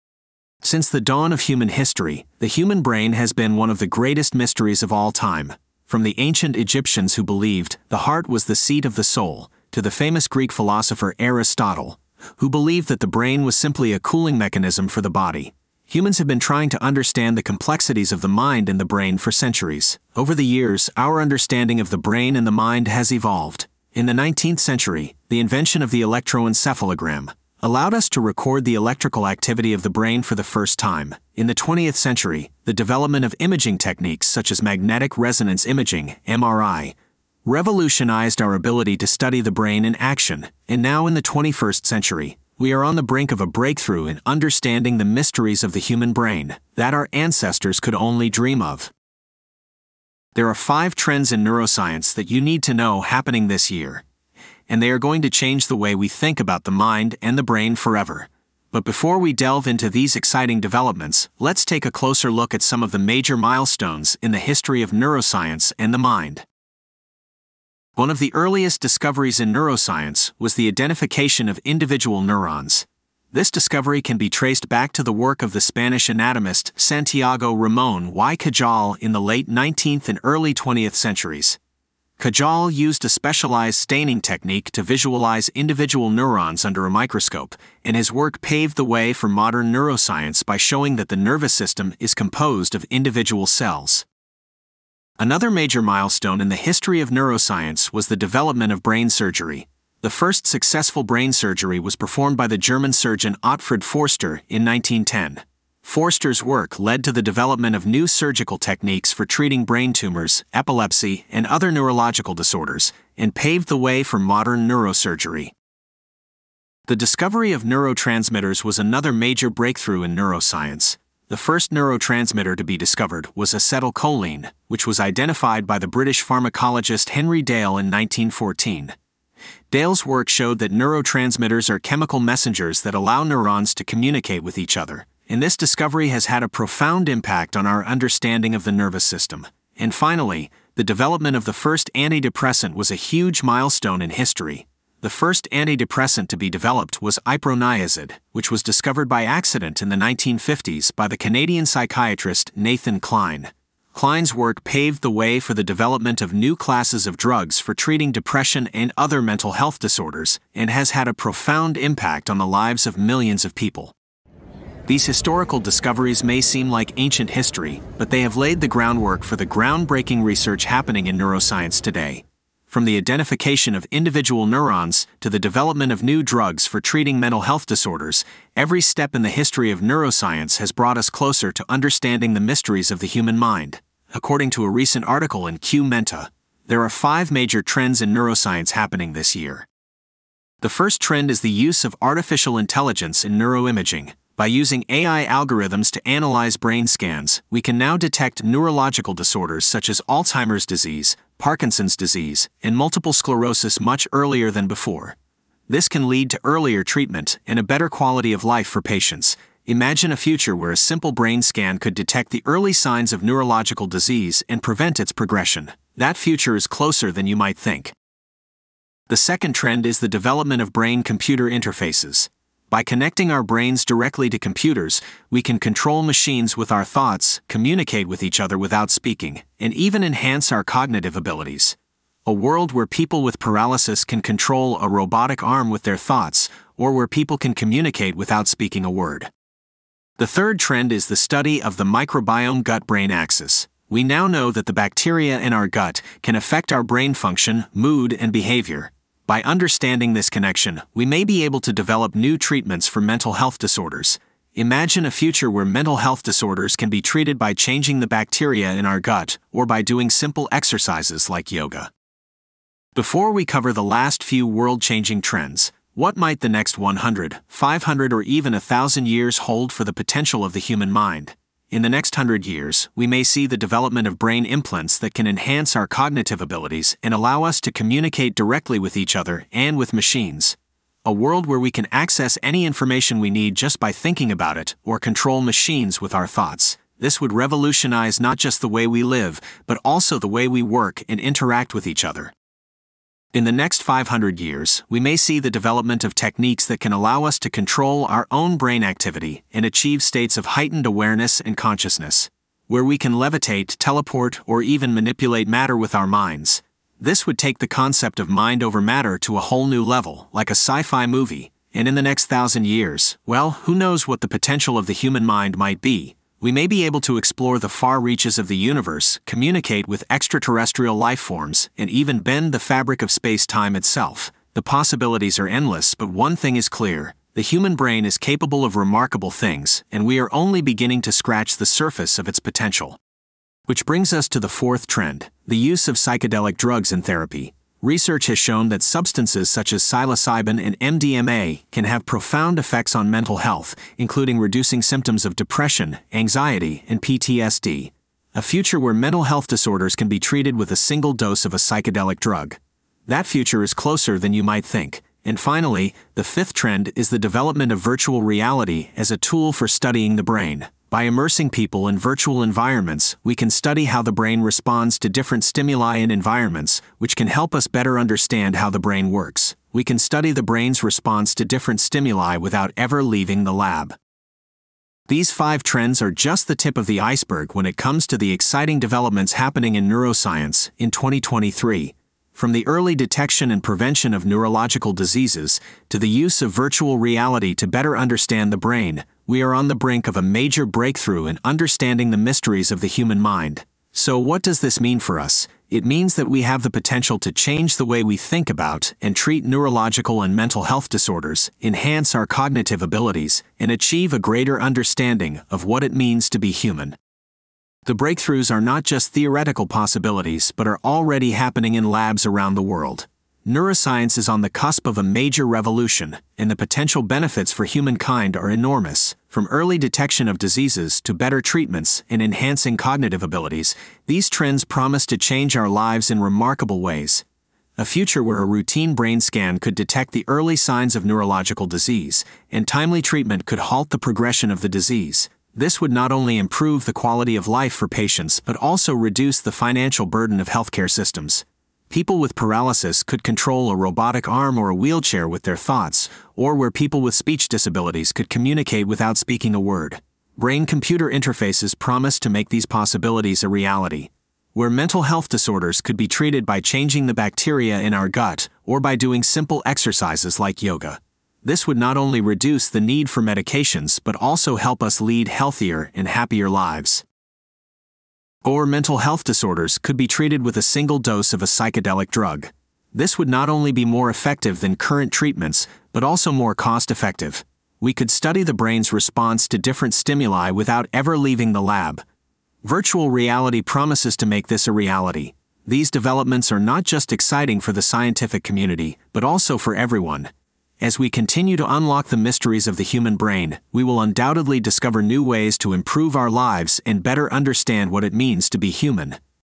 Top 5 Breakthroughs of 2023 : Revolutionizing Neuroscience
World Changing breakthroughs happening this year!
Are you ready to delve into the cutting-edge developments in neuroscience and the mind that are set to revolutionize our understanding of the human brain? Join us on a journey of discovery as we explore the latest trends in neuroscience that promise to unlock the full potential of the human mind in 2023 and beyond.
From brain-computer interfaces to psychedelic therapy, the top five breakthroughs in neuroscience that you need to know in 2023 offer new hope for treating neurological and mental health disorders. We begin our exploration with the study of the microbiome-gut-brain axis, which is revolutionizing the way we approach mental health treatment.

The microbiome is the collection of microorganisms that live in our gut, and research has shown that it has a direct impact on our brain function, mood, and behavior. By understanding how the microbiome interacts with the brain, we may be able to develop new treatments for mental health disorders, such as depression, anxiety, and autism.
Virtual reality is another exciting development in the field of neuroscience, and it is transforming the way we study the brain. VR allows researchers to simulate complex scenarios and environments that are difficult to replicate in the real world, providing a powerful tool for studying brain function and developing new treatments for neurological and mental health disorders.
Brain implants represent another exciting area of development in neuroscience, and they have the potential to enhance our cognitive abilities and allow us to communicate directly with machines. Brain-computer interfaces have already been used successfully to help patients with paralysis to control prosthetic limbs, and researchers are exploring other potential applications, such as restoring vision and hearing.
Psychedelic therapy is also making a comeback, as researchers explore the potential of drugs like psilocybin and MDMA to treat depression, anxiety, and PTSD. These drugs are not without risks, but preliminary studies have shown promising results, and researchers are hopeful that they may represent a new frontier in mental health treatment.
As we explore these exciting developments in neuroscience, it's important to remember the historical milestones that have paved the way for modern neuroscience. For example, the identification of individual neurons by the Spanish anatomist Santiago Ramón y Cajal in the early 20th century was a major breakthrough, as it showed that the nervous system is composed of individual cells. The development of brain surgery by the German surgeon Otfrid Foerster in 1910 also represented a major milestone, as it led to the development of new surgical techniques for treating brain tumors, epilepsy, and other neurological disorders.
The discovery of neurotransmitters was another major breakthrough in neuroscience, and it has had a profound impact on our understanding of the nervous system. The first neurotransmitter to be discovered was acetylcholine, which was identified by the British pharmacologist Henry Dale in 1914. Dale's work showed that neurotransmitters are chemical messengers that allow neurons to communicate with each other, and this discovery has had a profound impact on our understanding of the nervous system.
Finally, the development of the first antidepressant was a major milestone in the history of neuroscience. The first antidepressant to be developed was iproniazid, which was discovered by accident in the 1950s by the Canadian psychiatrist Nathan Kline. Kline's work paved the way for the development of new classes of drugs for treating depression and other mental health disorders, and has had a profound impact on the lives of millions of people.
The potential of the human mind is truly limitless, and the developments we have discussed today are just the beginning. In the next hundred years, we may see the development of brain implants that can enhance our cognitive abilities and allow us to communicate directly with machines. In the next five hundred years, we may see the development of techniques that can allow us to control our own brain activity and achieve states of heightened awareness and consciousness. And in the next thousand years, who knows what breakthroughs in neuroscience and the mind we may achieve? We may unlock the secrets of the brain and discover new frontiers of human experience that we can't even imagine today.